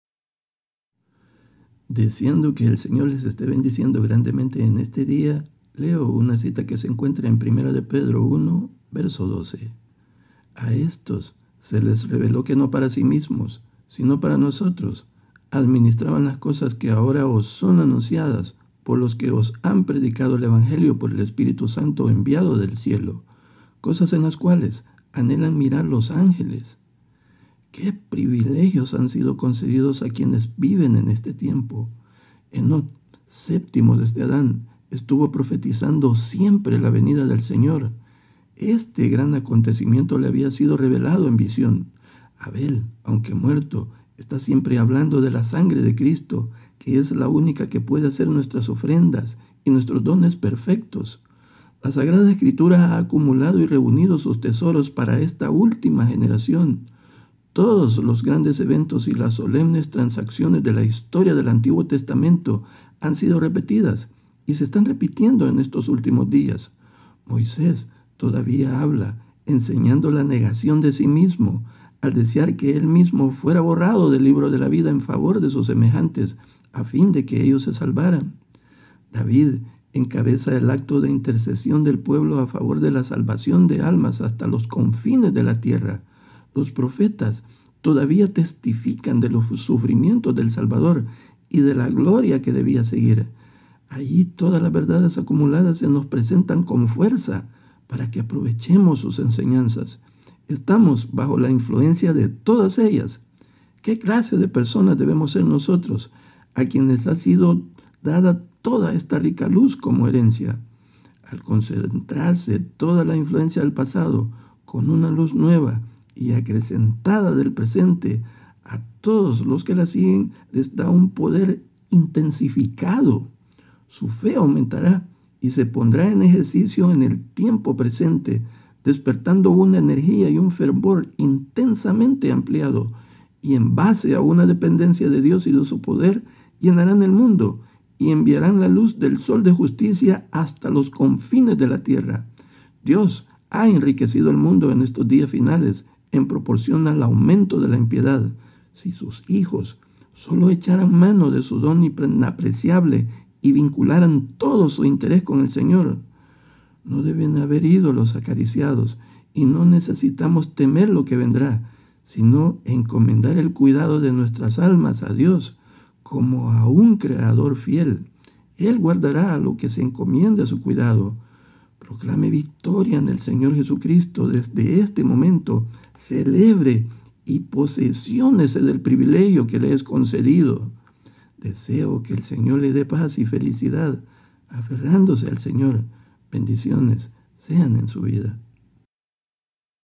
TESOROS PARA LA ÚLTIMA GENERACIÓN. 27 de marzo 2023.
Deseando que el Señor les esté bendiciendo grandemente en este día. Leo una cita que se encuentra en 1 Ped 1 verso 12, “A estos se les reveló que no para sí mismos, sino para nosotros, administraban las cosas que ahora os son anunciadas por los que os han predicado el evangelio por el Espíritu Santo enviado del cielo; cosas en las cuales anhelan mirar los ángeles”
¡Qué privilegios han sido concedidos a quienes viven en este tiempo! Enoc, séptimo desde Adán, estuvo profetizando siempre la venida del Señor. Este gran acontecimiento le había sido revelado en visión. Abel, aunque muerto, está siempre hablando de la sangre de Cristo, que es la única que puede hacer nuestras ofrendas y nuestros dones perfectos. La Sagrada Escritura ha acumulado y reunido sus tesoros para esta última generación. Todos los grandes eventos y las solemnes transacciones de la historia del Antiguo Testamento han sido repetidas y se están repitiendo en estos últimos días. Moisés todavía habla, enseñando la negación de sí mismo al desear que él mismo fuera borrado del libro de la vida en favor de sus semejantes, a fin de que ellos se salvaran.
David encabeza el acto de intercesión del pueblo a favor de la salvación de almas hasta los confines de la tierra. Los profetas todavía testifican de los sufrimientos del Salvador y de la gloria que debía seguir. Allí todas las verdades acumuladas se nos presentan con fuerza para que aprovechemos sus enseñanzas. Estamos bajo la influencia de todas ellas. ¿Qué clase de personas debemos ser nosotros, a quienes ha sido dada toda esta rica luz como herencia? Al concentrarse toda la influencia del pasado con una luz nueva y acrecentada del presente, a todos los que la siguen se les da un poder intensificado.
Su fe aumentará y se pondrá en ejercicio en el tiempo presente, despertando una energía y un fervor intensamente ampliado; y en base a una dependencia de Dios y de su poder, llenarán el mundo y enviarán la luz del Sol de Justicia hasta los confines de la tierra. Dios ha enriquecido el mundo en estos días finales en proporción al aumento de la impiedad, si sus hijos sólo echaran mano de su don inapreciable y vincularan todo su interés con el Señor. No debe haber ídolos acariciados, y no necesitamos temer lo que vendrá, sino encomendar el cuidado de nuestras almas a Dios, como a un Creador fiel.
El guardará lo que se encomiende a su cuidado. Proclame victoria en el Señor Jesucristo, desde este momento, celebre y posesiónese del privilegio que le es concedido. Deseo que el Señor le de paz y felicidad, aferrándose al Señor. Bendiciones sean en su vida.